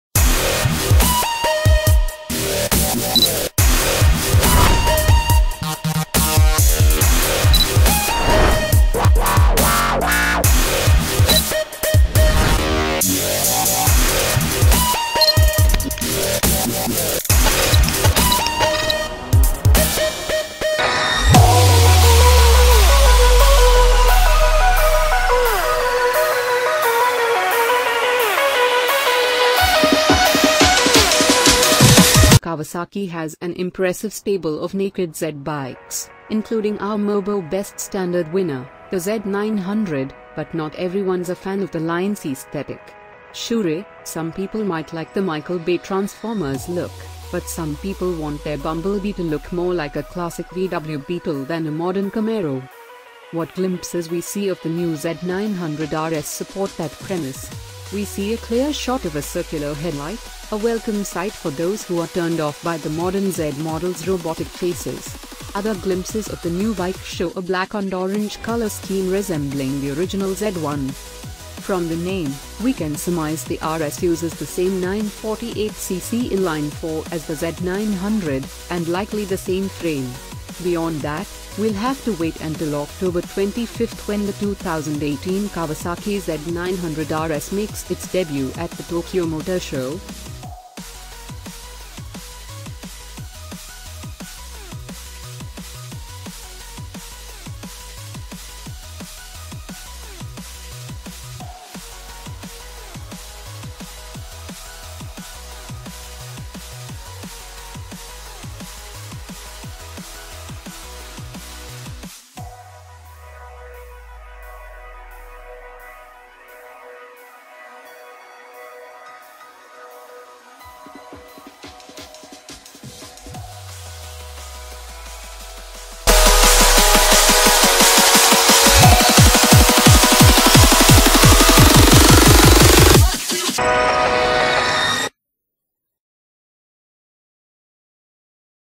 HOT NEWS !!! 2018 new Kawasaki Z900RS PRICE & SPEC
HOT NEWS !!! 2018 new Kawasaki Z900RS PRICE & SPEC - Kawasaki has an impressive stable of naked Z bikes (including our MOBO Best Standard winner, the Z900) but not everyone’s a fan of the line’s aesthetic. Sure, some people might like the Michael Bay Transformers look, but some people want their Bumblebee to look more like a classic VW Beetle than a modern Camaro.